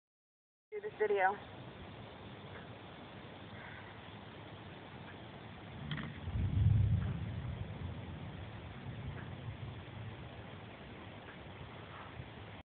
Police Dodge Charger Spotlight
Outfitted and sold by Martel Emergency Vehicle Solutions (EVS) AKA Martel Autos.